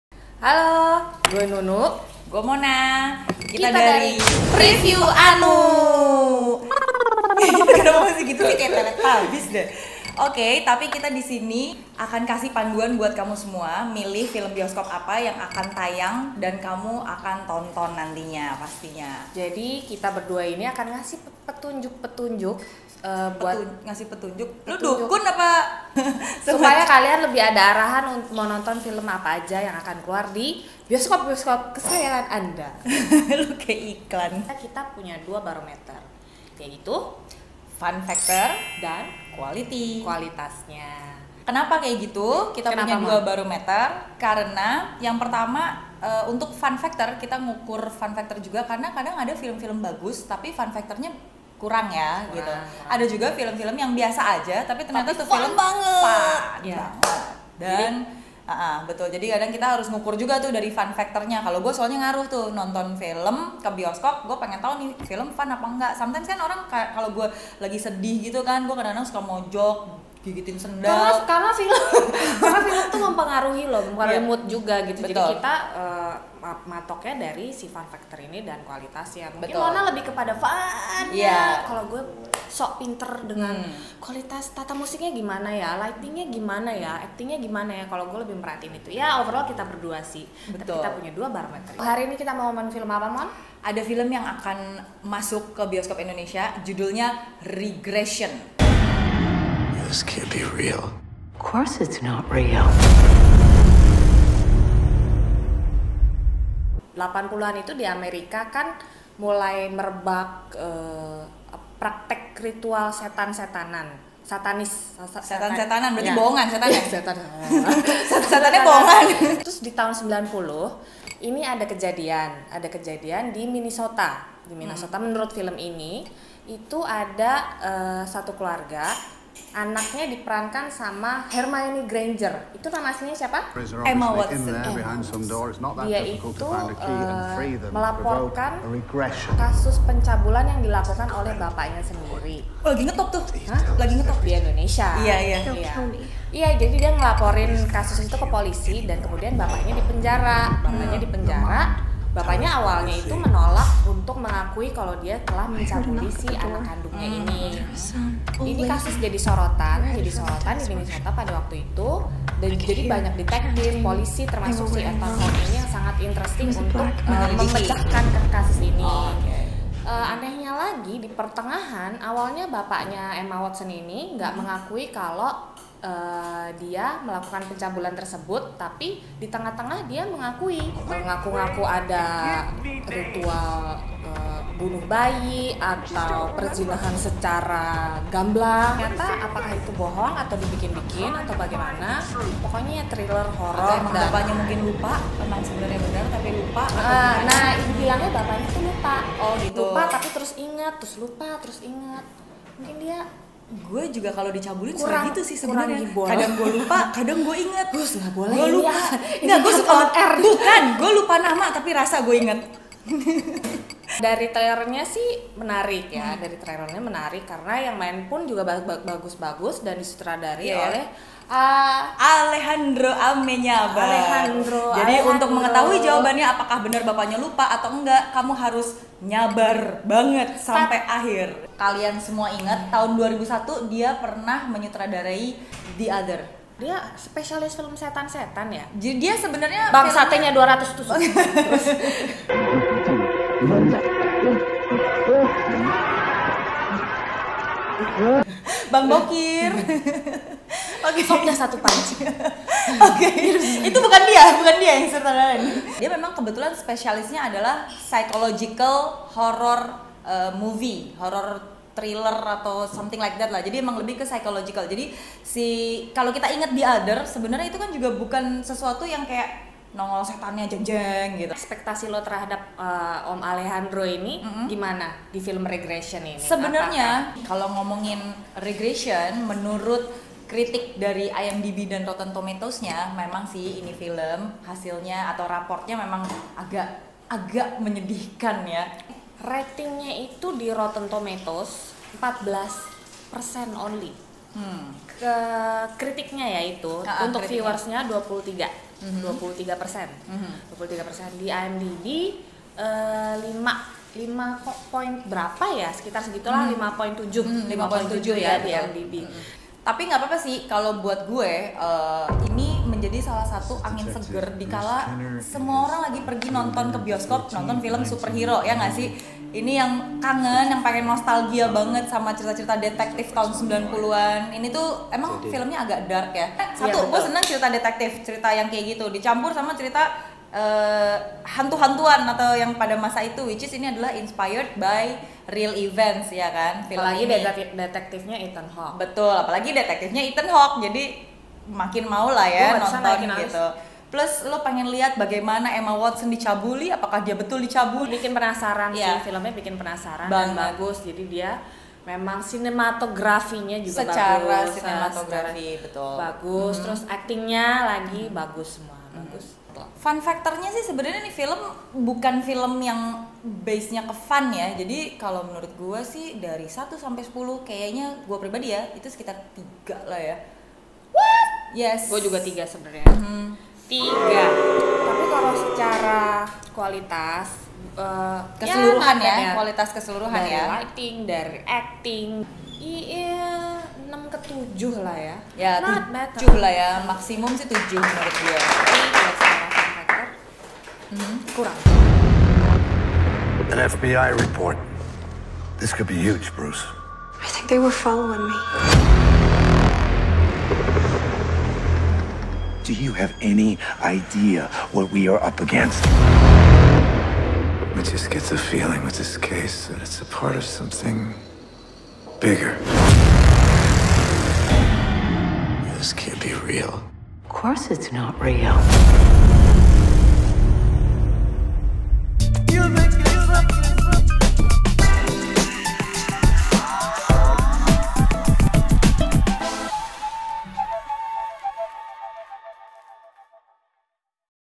PREVIEW ANU: REGRESSION
segera beredar di bioskop Indonesia bulan Mei-Juni 2016. tonton Preview Anu dulu sebelum memustuskan nonton apa jangan.

Regression
genre: horror, thriller, drama
pemain: Ethan Hawk, Emma Watson
sutradara: Alejandro Amenabar

rating per 24 mei 2016
IMDB rating: 5,7
tomatoers: 16%
rotten tomatoes user: 23%